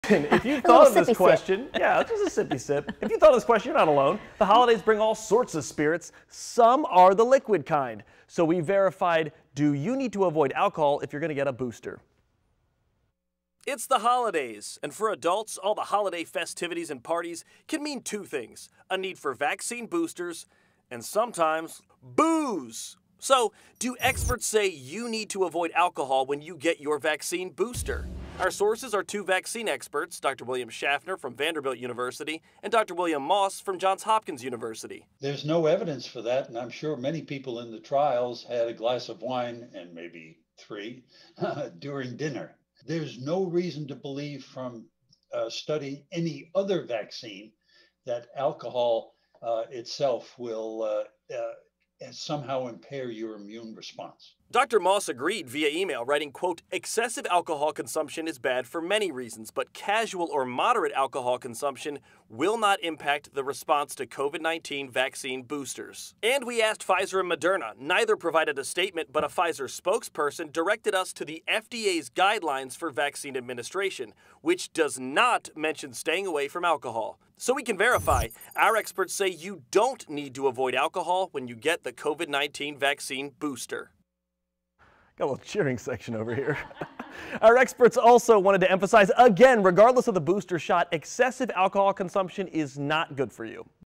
Drinking alcohol won't affect your COVID-19 booster shot
Social media posts claim booze and boosters don't mix. It's not true.